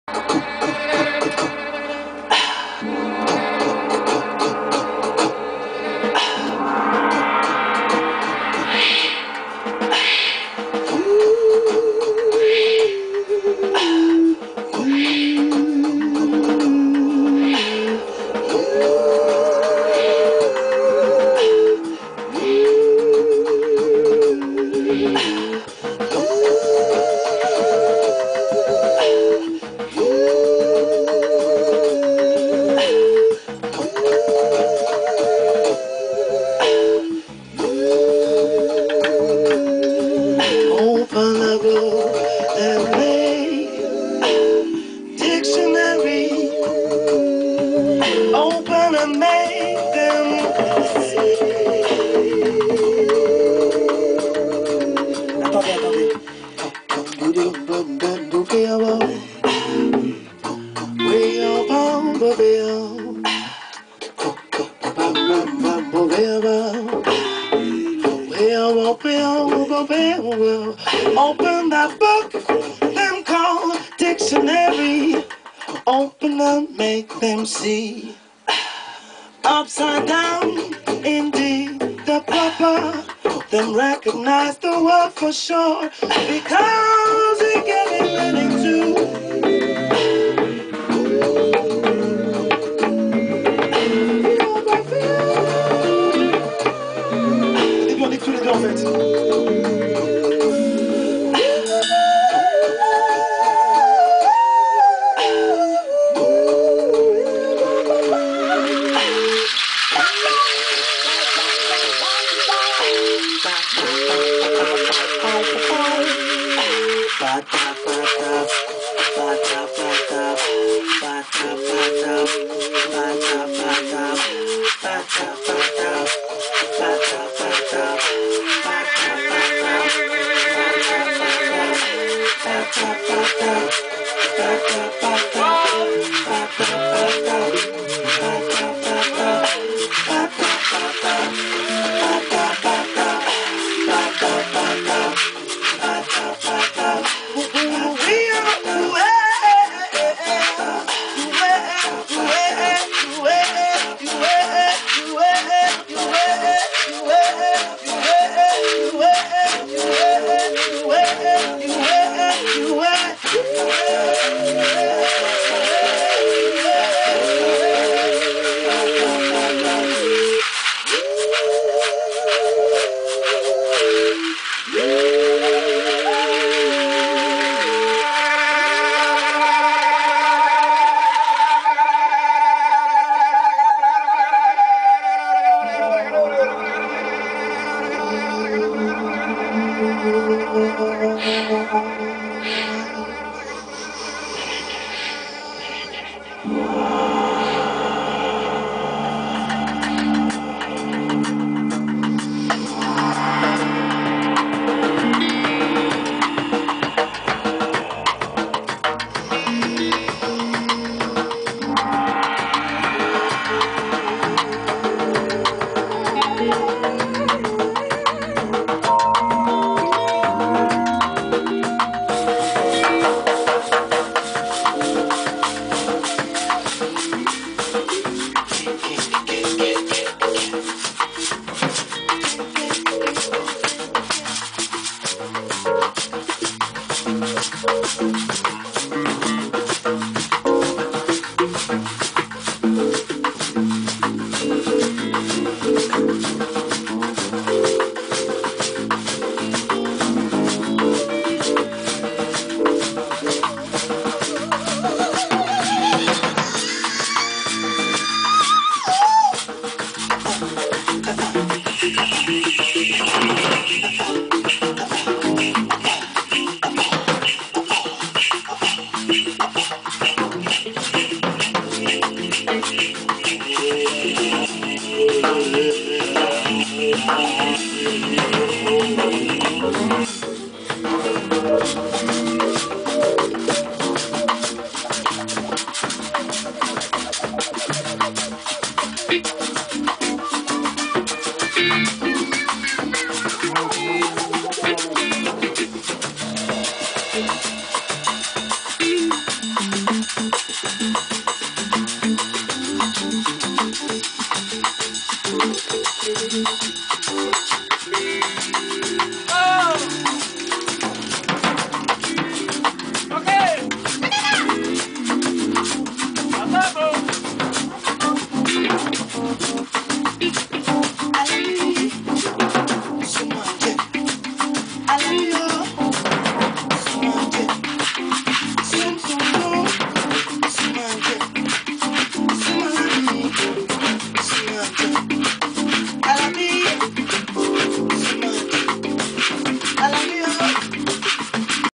Estação SESC Brasil - Ensaio de Naná Vasconcelos com Sandra Nkaké
A sintonia de Naná Vasconcelos com Sandra Nkaké é algo indescritível. Os dois se apresentam amanhã, no Estação SESC Brasil (Station Brésil), no SESC Pinheiros.